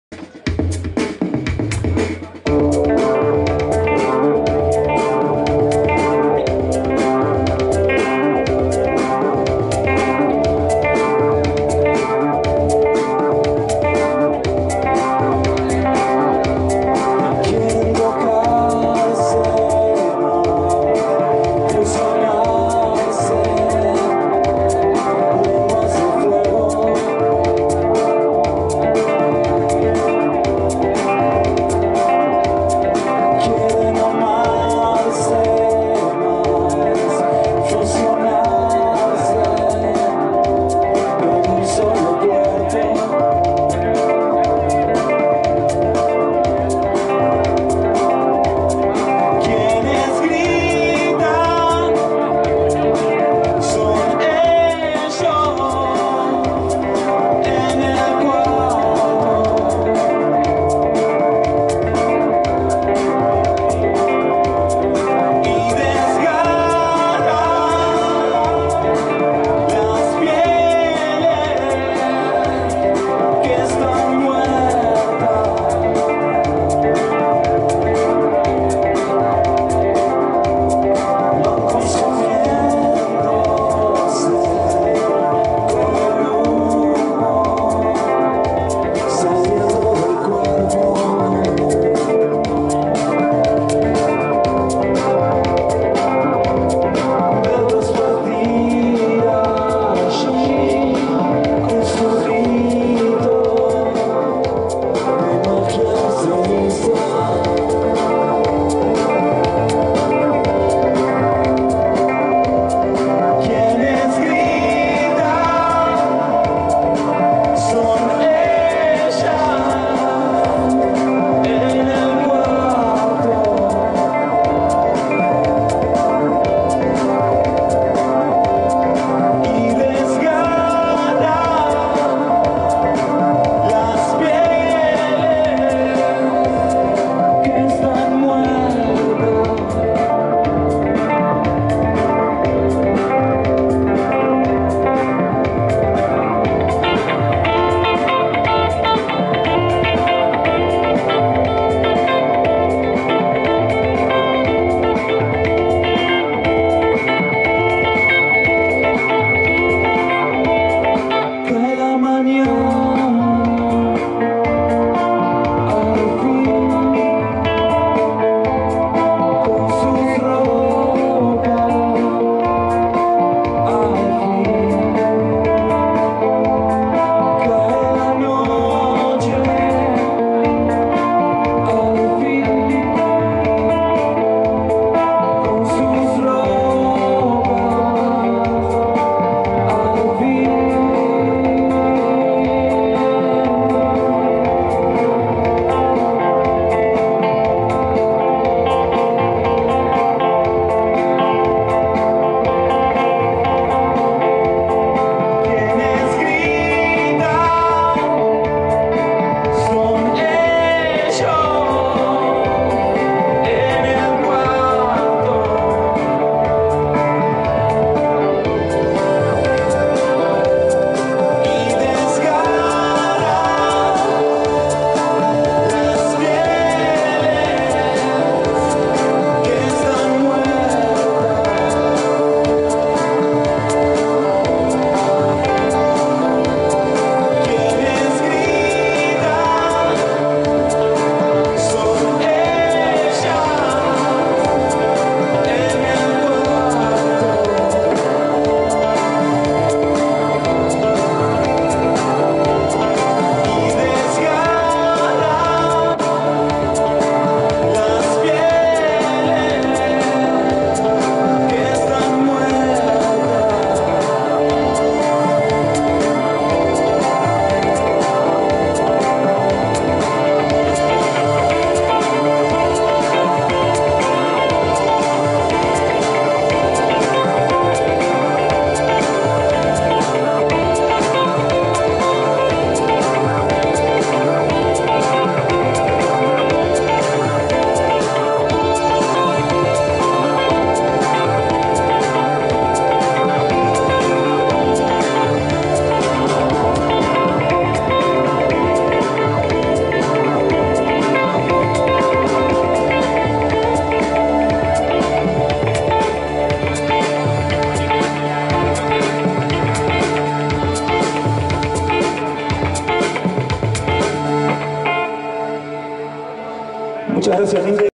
Las Fuerzas Extrañas - 17/11/2017
Lucas Vanza - Guitarras, voces
Gaston Paganini - Bajo
Benjamin Riderelli - Drum machine, Programaciones